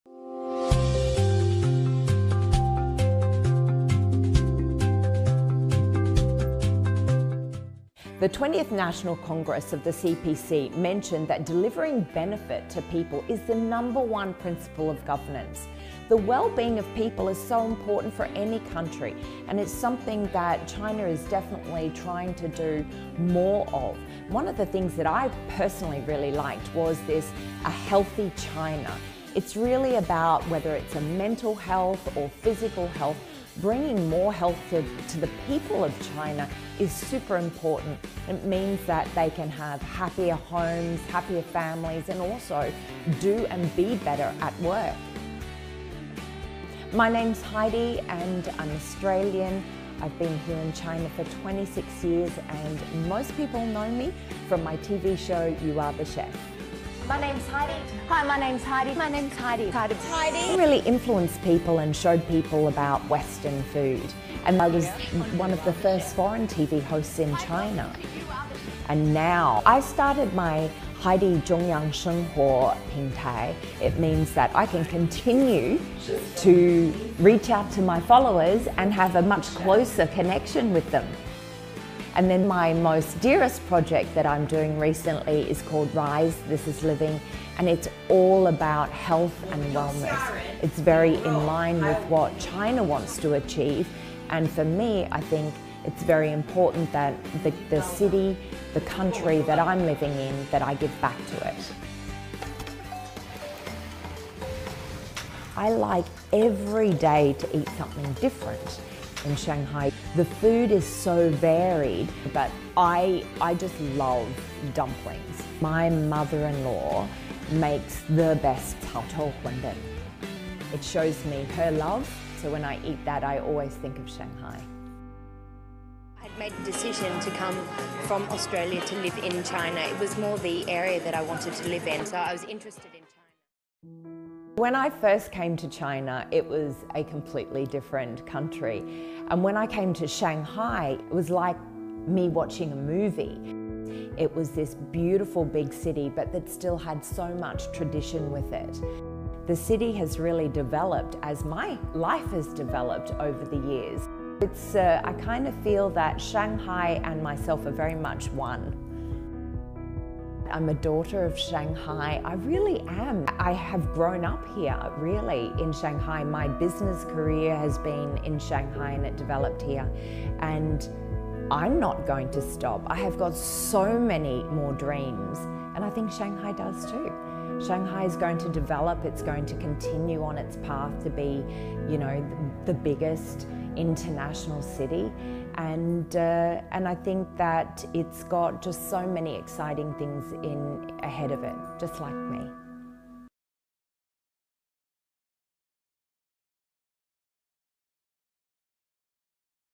老外讲故事·另眼观盛会（12）| 海蒂：作为“上海的女儿”，我的台前幕后都在这里演绎“理想人生”
Heidi Dugan出生于澳大利亚，却已在中国生活了26年，是中国最早一批外籍主持人之一，所主持的《洋厨房》节目影响了几代人。